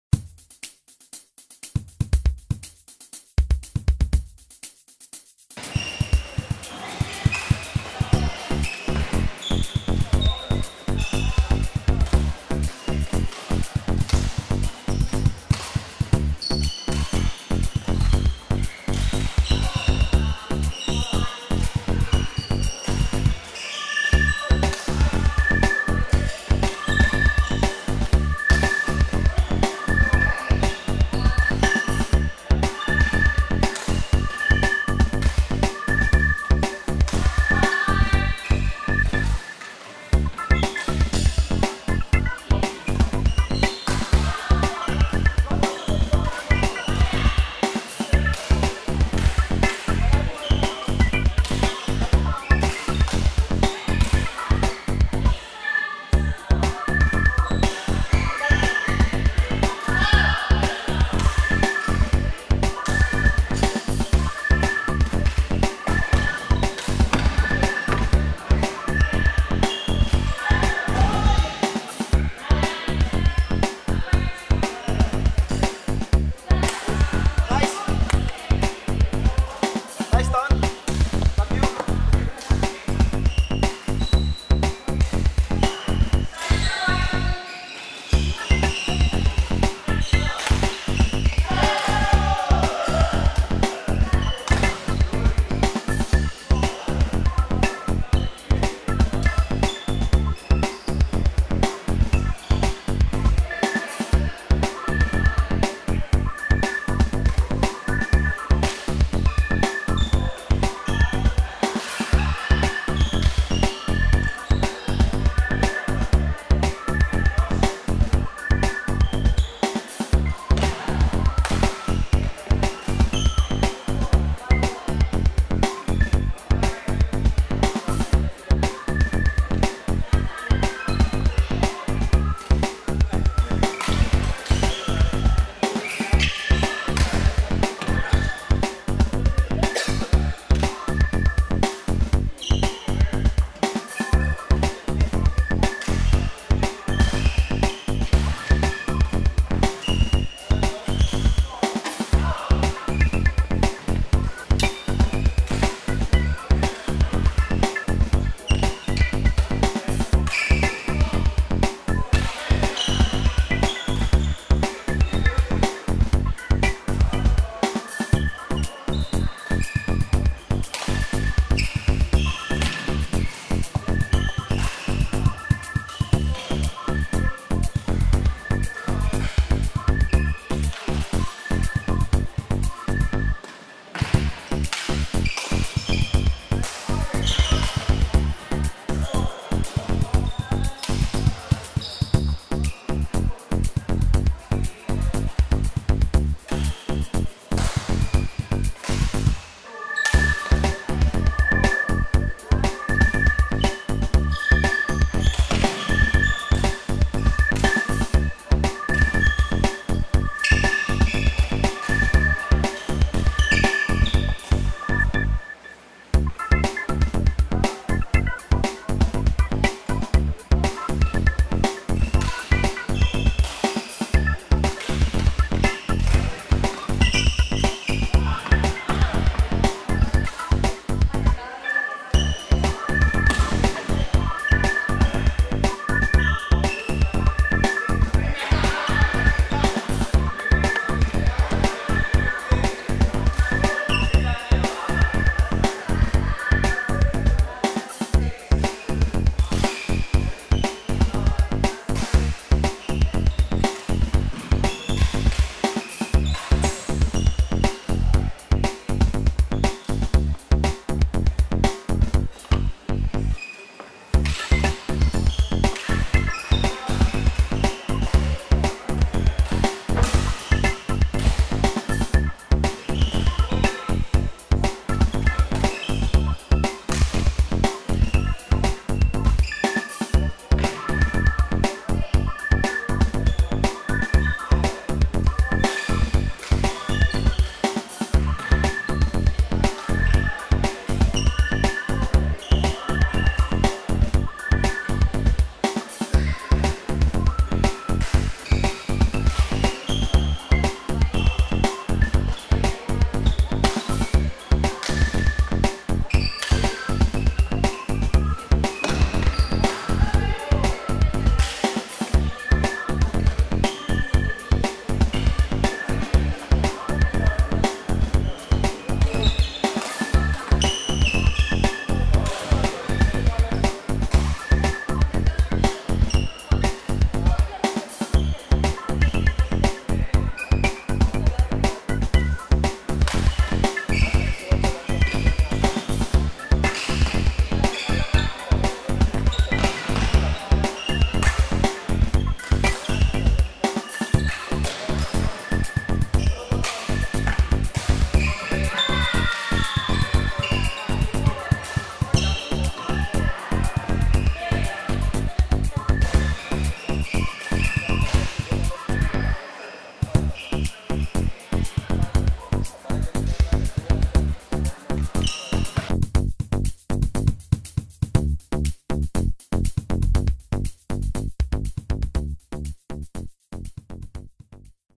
Ricci and Tonton First Match with Fred and Paul
Ricci and Tonton First Match with Fred and Paul at the 2nd Sunday Smashers Tourney 2009. Held at Bedok South Secondary School.